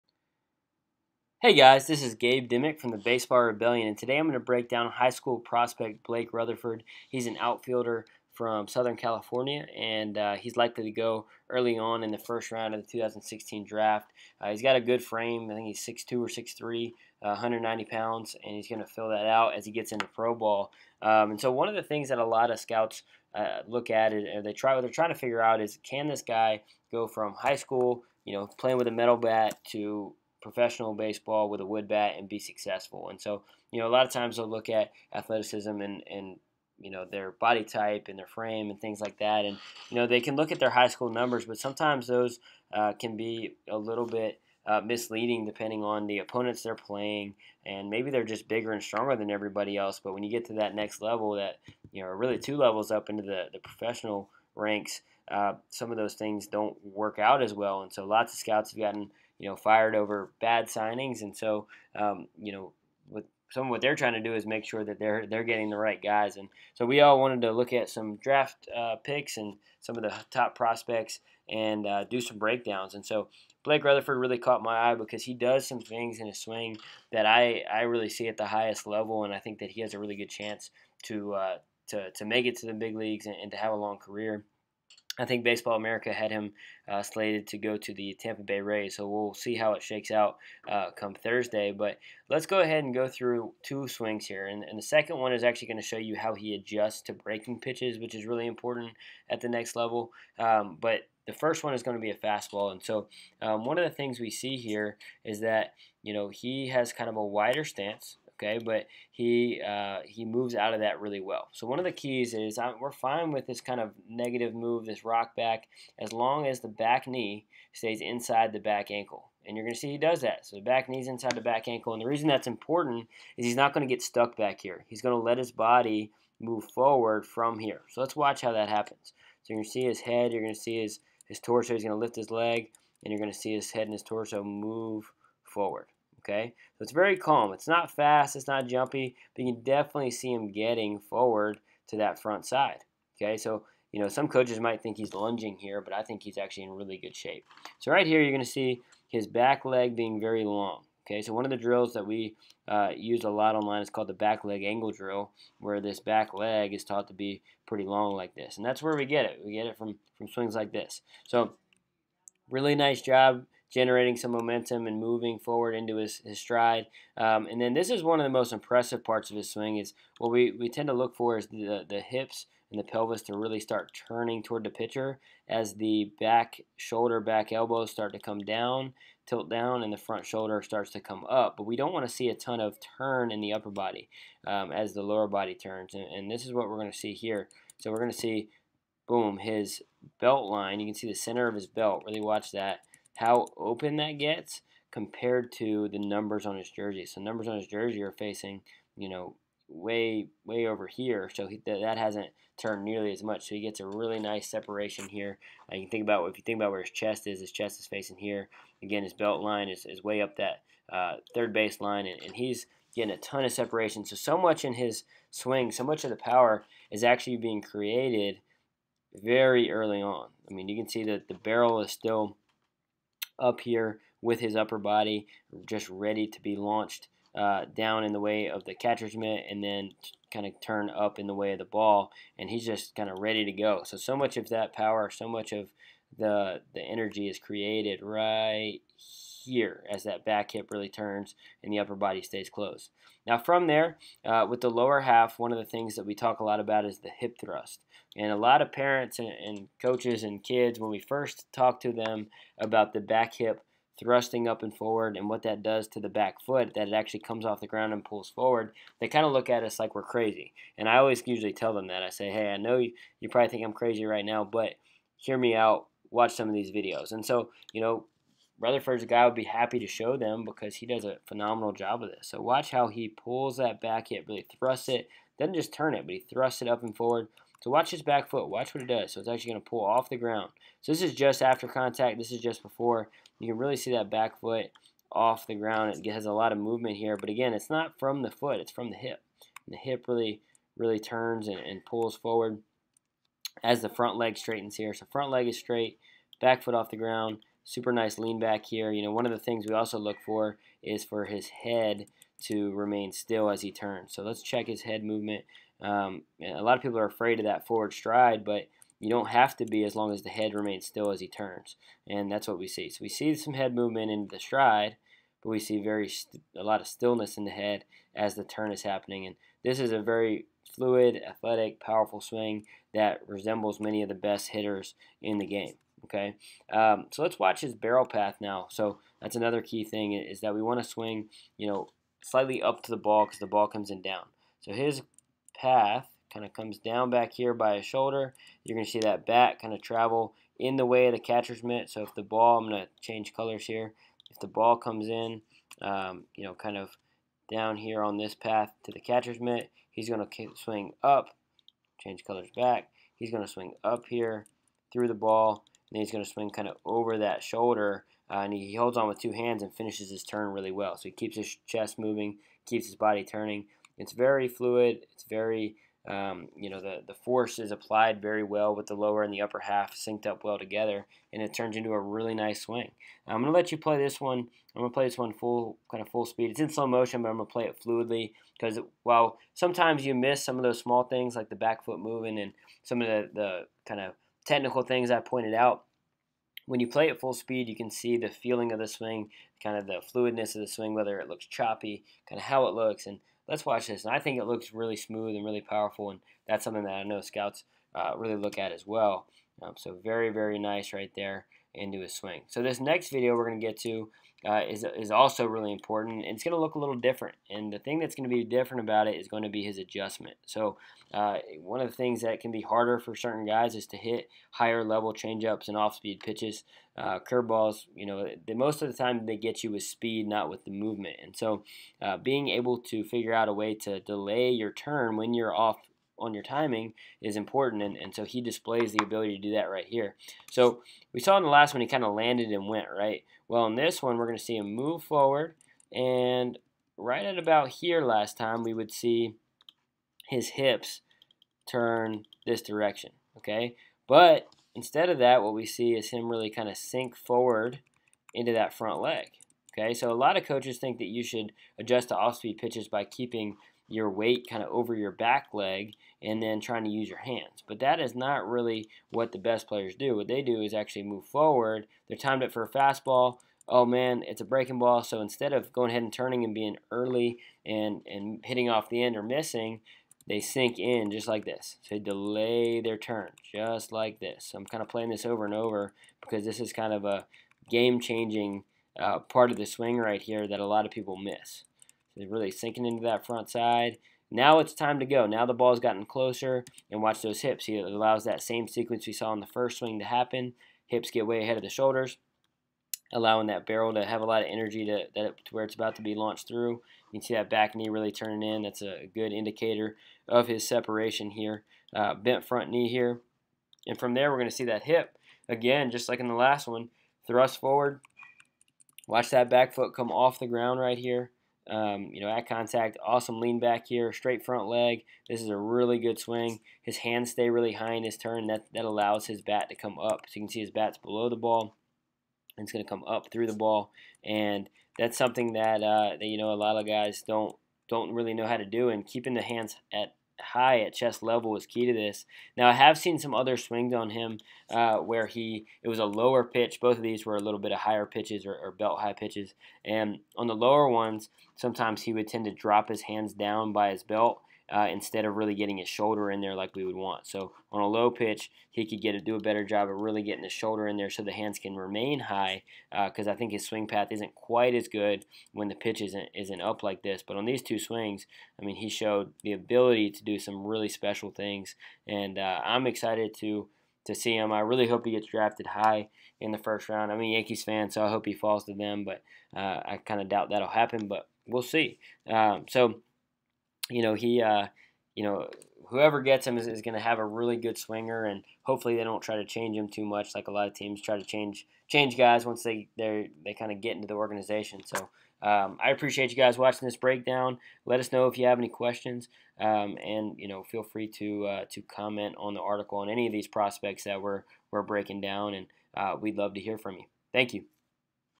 Blake Rutherford: Baseball Rebellion Swing Breakdown
Baseball Rebellion breaks down the swing of high school prospect Blake Rutherford. His smooth movements along with his ability to separate the lower and upper halves extremely well allow him to display a polished swing that should produce both power and accuracy.